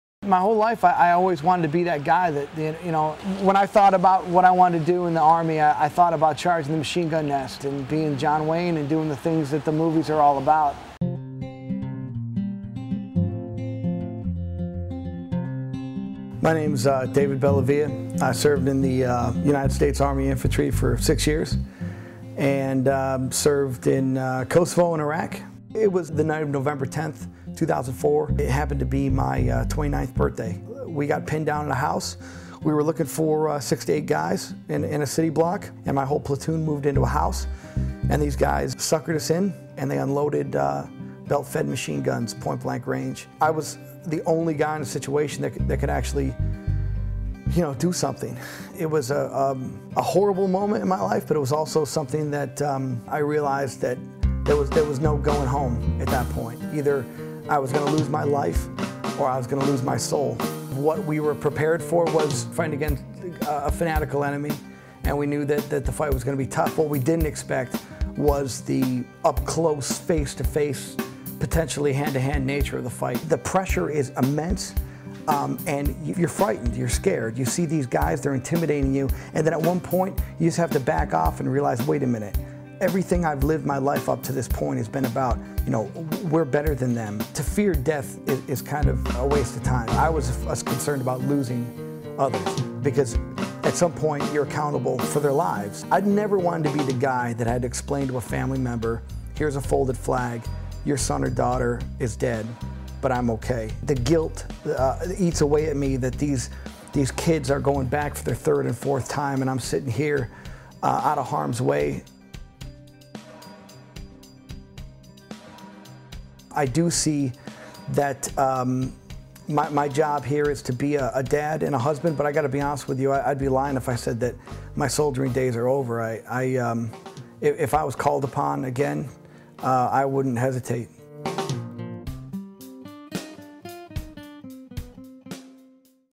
War stories
Bringing to searing life the terrifying intimacy of hand-to-hand infantry combat, House to House is far more than just another war story. Populated by an indelibly drawn cast of characters, from a fearless corporal who happens to be a Bush-hating liberal to an inspirational sergeant-major who became the author's own lost father figure, it develops the intensely close relationships that form between soldiers under fire. Their friendships, tested in brutal combat, would never be quite the same. Not all of them would make it out of the city alive. What happened to them in their bloody embrace with America's most implacable enemy is a harrowing, unforgettable story of triumph, tragedy, and the resiliency of the human spirit. Staff Sergeant David Bellavia spent six years in the U.S. Army, including some of the most intense fighting of the Iraq War. He has been awarded the Silver Star and Bronze Star for his actions in Iraq, and recommended for the Distinguished Service Cross and Medal of Honor for his actions in Fallujah. In 2005, he received the Conspicuous Service Cross (New York State's highest award for military valor) and was inducted into the New York State Senate Veterans Hall of Fame. He is the cofounder of Vets for Freedom, an advocacy organization of veterans concerned about the politicization of media coverage of military operations in Iraq and Afghanistan. His writing has been published in The Philadelphia Inquirer, National Review, The Weekly Standard, and other publications. He lives in western New York.